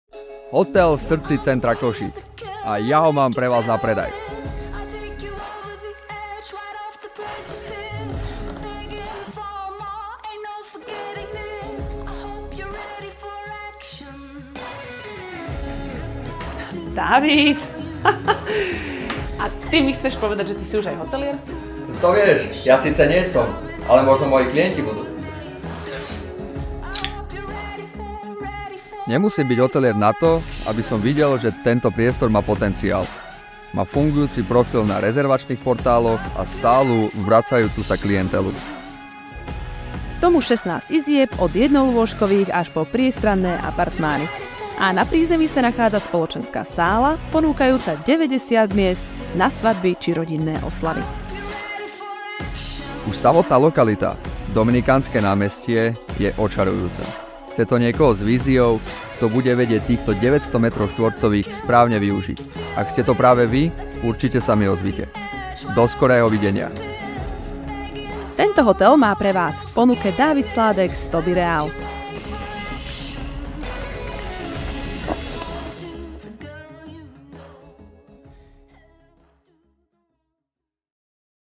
Hotel Zlaté Košice v srdci mesta Košíc na Dominikánskom námestí
***PREDAJ***

Hľadáte priestor, ktorý nebude len Vašim biznisom, ale aj Vašou srdcovou záležitosťou? Predstavujeme Vám jedinečnú nehnuteľnosť, ktorá ponúka komfortné ubytovanie, modernú reštauráciu a strategickú polohu pre Váš prosperujúci biznis.

👉úžitková plocha takmer 984,73 m2
👉 15 izieb
👉 4 nadzemné podlažia
👉 mesačné náklady na prevádzku hotela (elektrina, plyn, voda) sú 3 000 €.
👉 celoročná obsadenosť

Dispozícia:

👉 1.PP (90,27 m2):
Poskytuje dostatok miesta pre sklady a technickú miestnosť, čo je ideálne pre logistiku a prevádzku hotela.

👉 1. NP (282,53 m2):
Srdcom tohto podlažia je štýlová a priestranná hlavná sála reštaurácie o rozlohe 163,7 m2 s kapacitou 90 osôb. Okrem kompletne vybavenej kuchyne sa tu nachádzajú aj šatne a toalety pre zamestnancov, a taktiež samostatné toalety pre dámy a pánov.

👉 2. NP (280,38 m2):
Na 2.NP nájdeme recepciu hotela, technickú miestnosť a 6 izieb s vlastnou kúpeľňou. Navyše tu nájdete veľký 3-izbový byt s kúpeľňou.

👉 3. NP (272,41 m2):
Ponúka kanceláriu, ďalšiu technickú miestnosť, práčovňu a 8 izieb s kúpeľňou.

👉 4. NP (59,14 m2):
Na poslednom nadzemnom podlaží sa nachádza veľký apartmán s rozlohou 55,96 m2.

💶  Cena je na vyžiadanie v RK.

Ak Vás táto ponuka zaujala, dohodnite si obhliadku s Dávidom Sládekom.